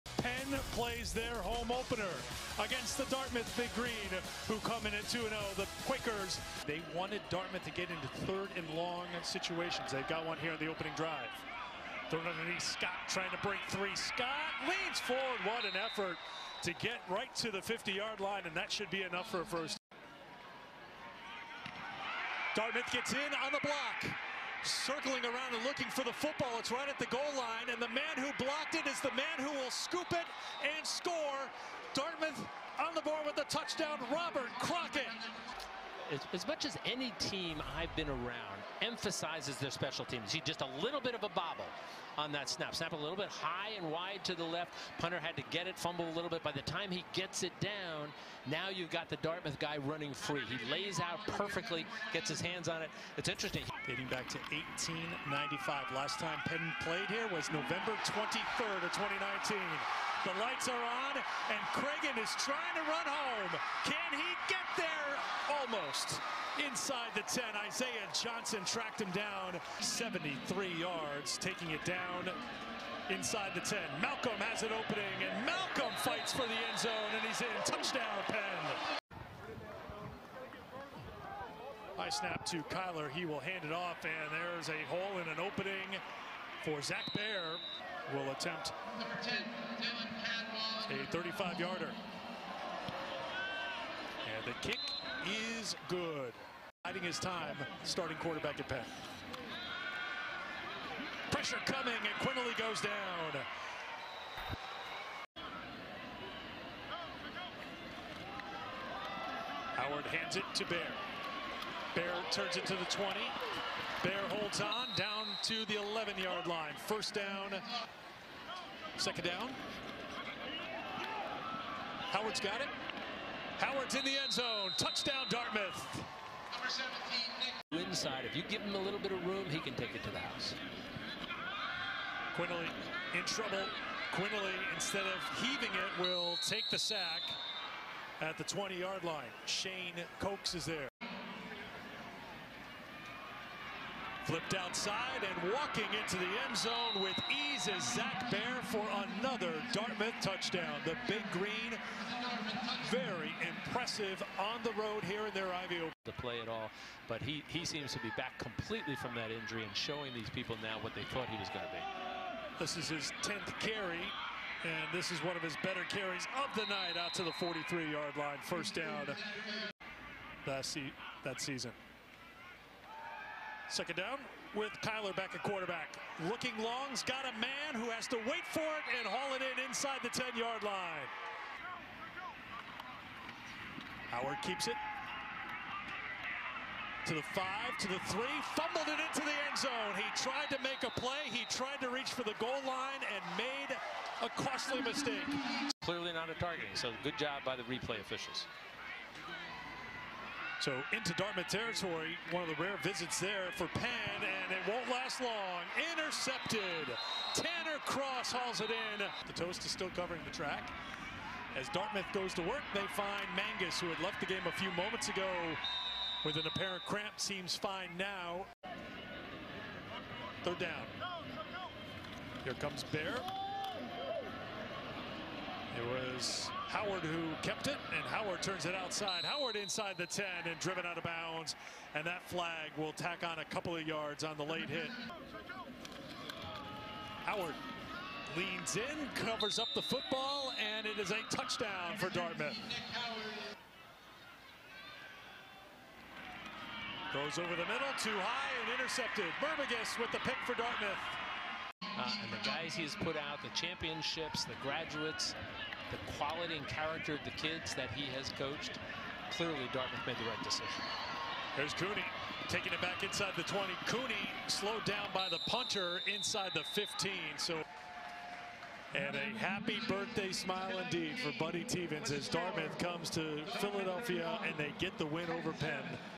Highlights: Football at Penn, Oct. 1, 2021
Some of the biggest plays from Dartmouth's Ivy League opener at Penn